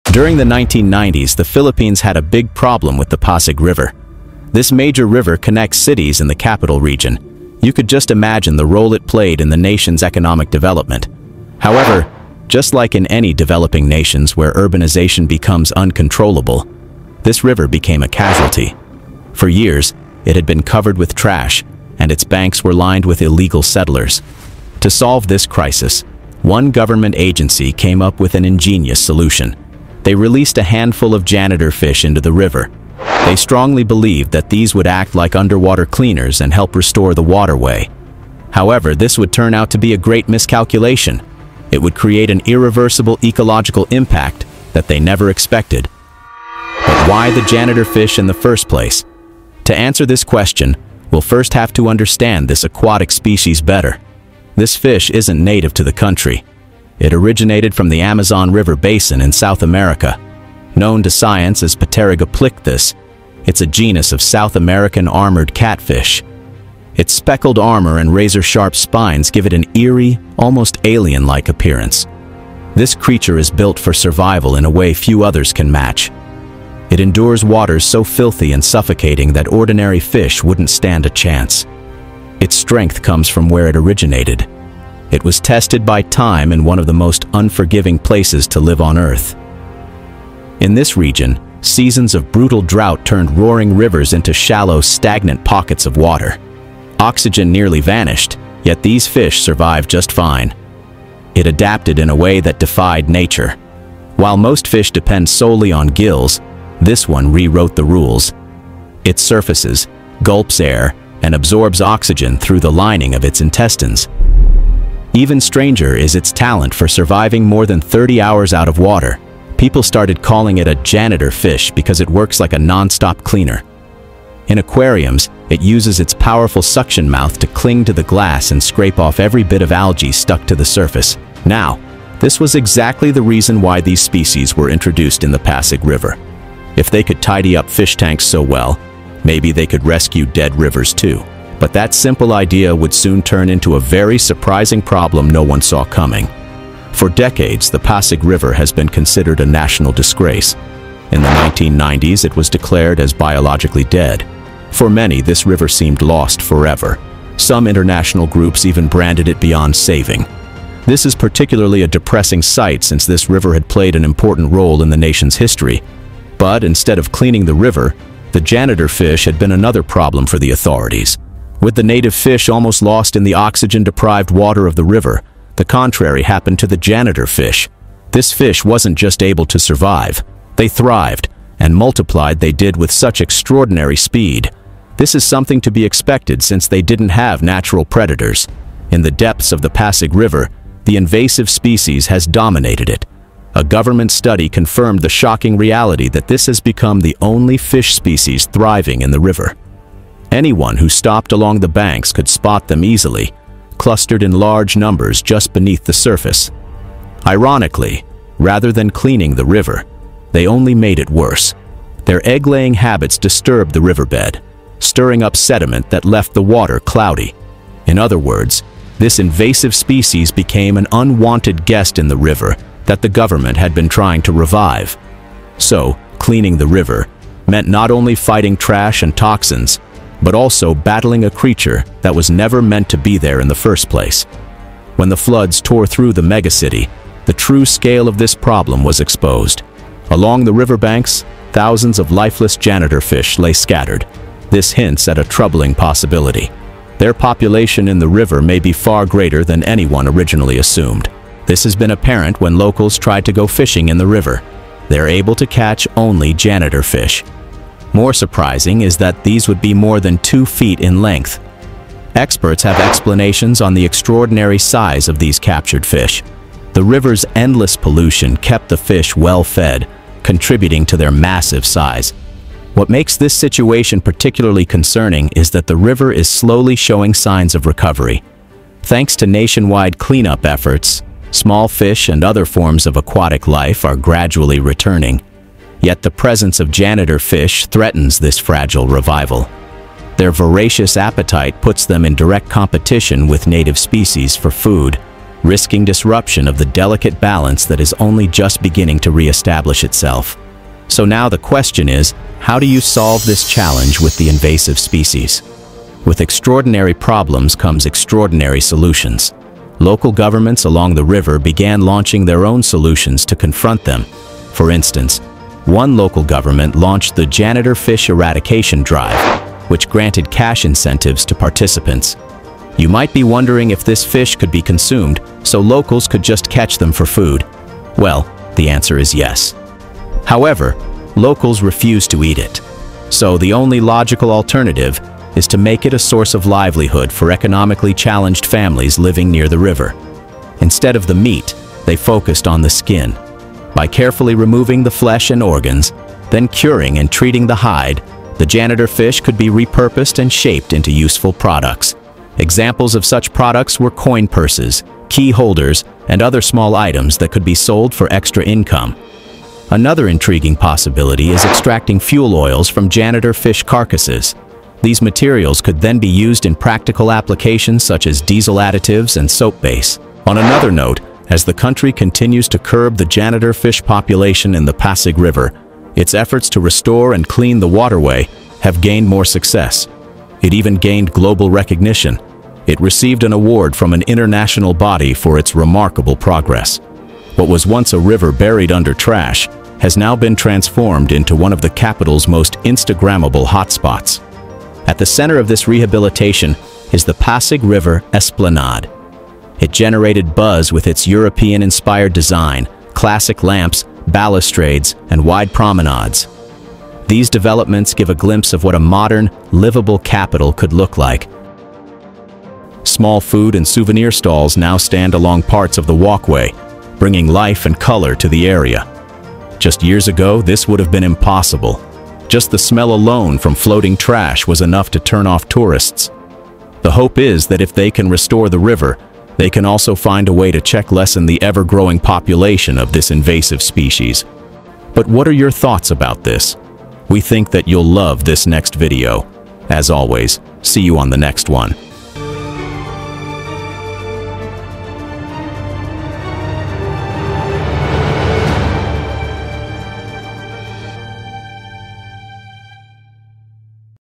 They Released Janitor Fish in the World's Dirtiest River...Then This Happened | Pasig River
In the 1990s, the Philippines had a big problem with the Pasig River. It was considered biologically dead.

To solve this crisis, one government agency came up with an ingenious solution.

They released a handful of janitor fish into the river. They strongly believed that these would act like underwater cleaners and help restore the waterway.

However, this would turn out to be a great miscalculation. It would create an irreversible ecological impact that they never expected.